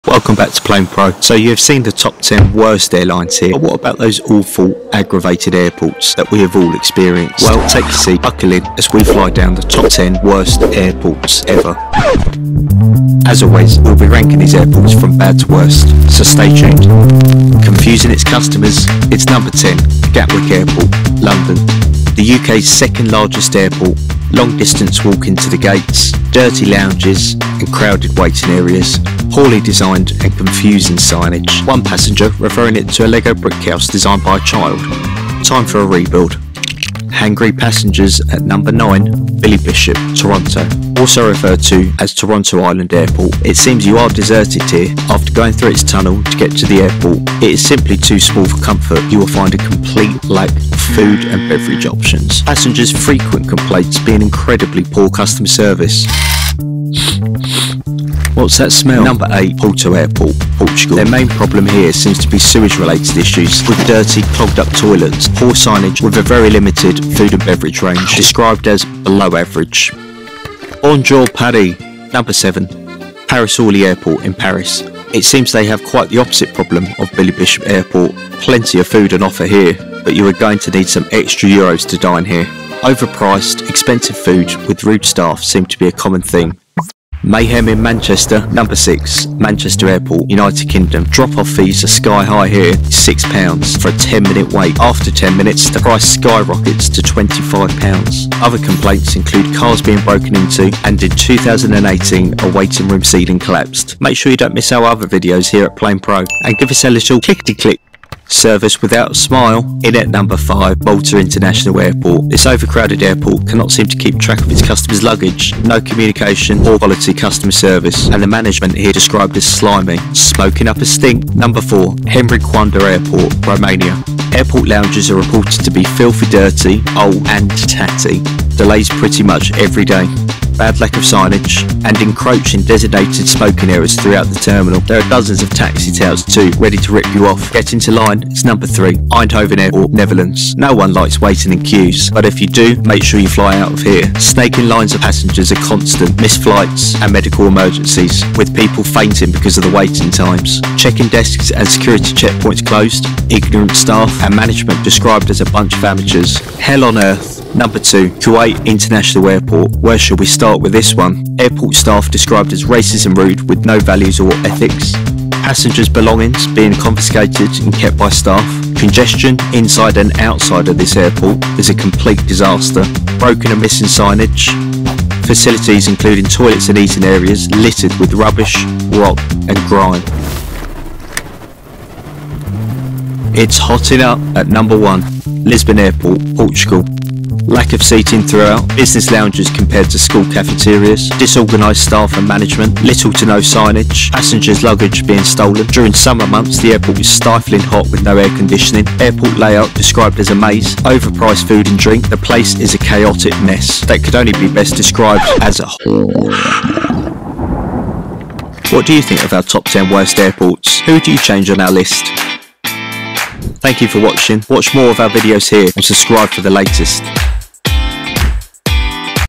Top 10 WORST Airports in the WORLD EVER!
Top 10 WORST Airports EVER!

Filthy terminals and rude staff.

We countdown the TOP 10 WORST AIRPORTS EVER.

From bad to WORST!
Confusing its customers, dirty lounges and confusing signage.
Small cramped waiting areas, smelly toilets and limited food and beverage range.
Poor customer service, over priced and expensive food and expensive drop off fees at airports. Over crowded and bad management. Old airports with taxi tout scams, long queues and medical emergencies. Staff racist and rude. Stolen luggage. Congestion around airport. Lack of seating and no air con. These are just some of the things covered in this video!

10.  Gatwick Airport (United Kingdom)
9. Billy Bishop (Toronto Island) Airport (Cananda)
8. Porto Airport (Portugal)
7. Paris Orly Airport (France)
6. Manchester Airport (United Kingdom)
5. Malta International Airport (Malta)
4. Henri Coanda Airport (Romania)
3. Eindhoven Airport (Netherlands)
2. Kuwait International Airport (Kuwait)
1. Lisbon Airport (Portugal)

What do you think about our list?